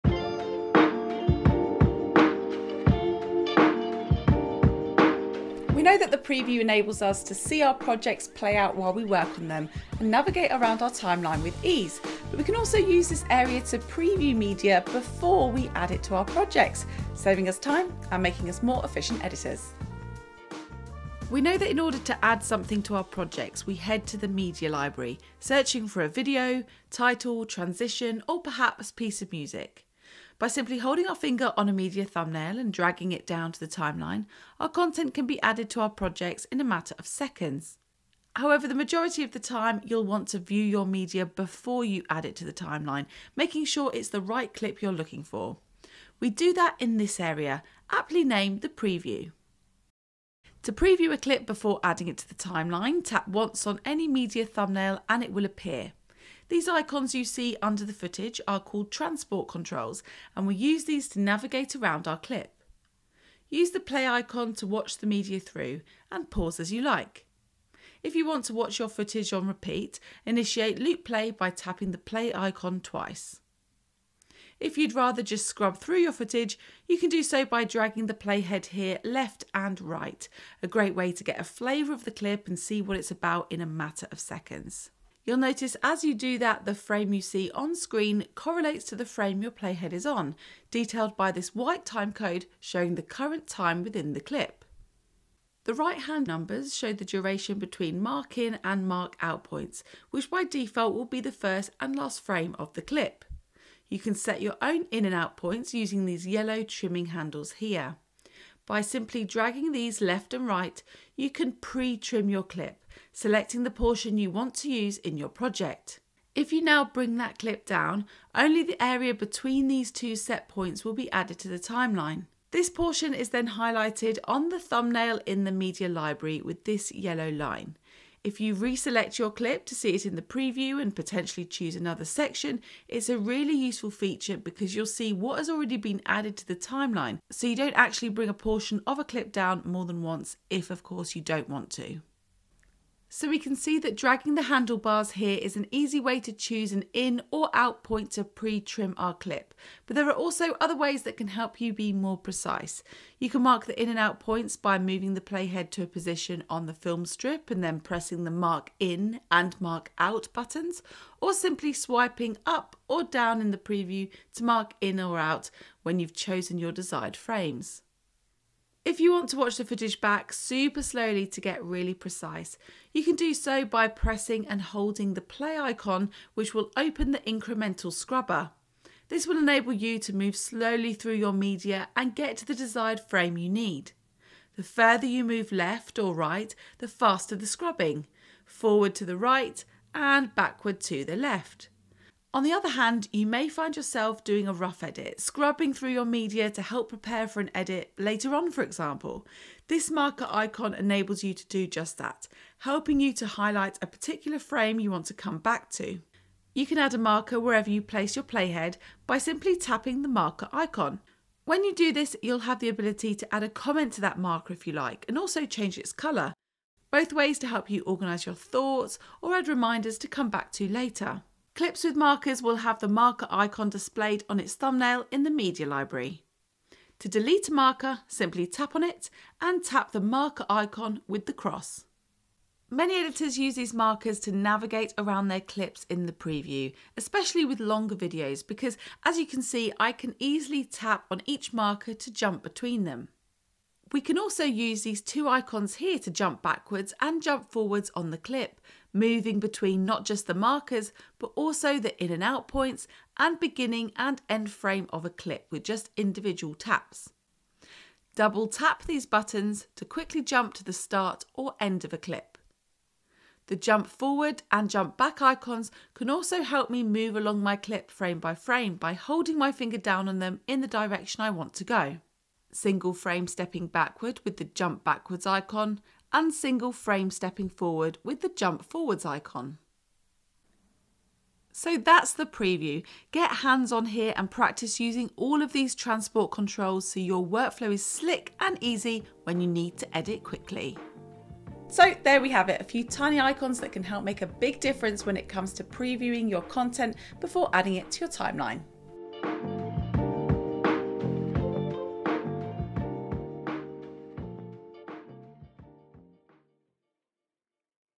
Previewing Your Media in LumaFusion
Did you know you can view, pre-trim and manage your media before you've even added it to the LumaFusion timeline? In this Reference Guide video, Caroline shows you how to make the most out of the Preview, exploring all the functions it offers to editors.